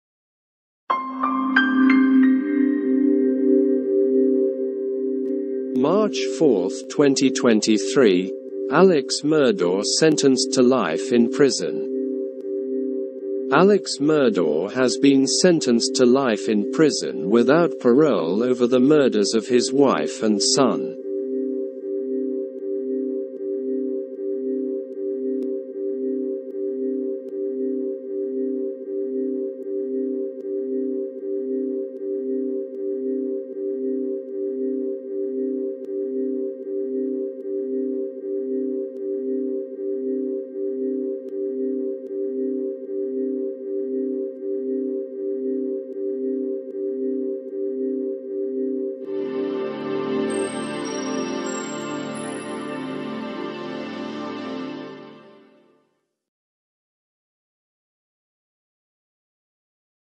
[Latest]Alex Murdaugh sentenced to life in prison
[Contents]
Alex Murdaugh has been sentenced to life in prison without parole over the murders of his wife and son.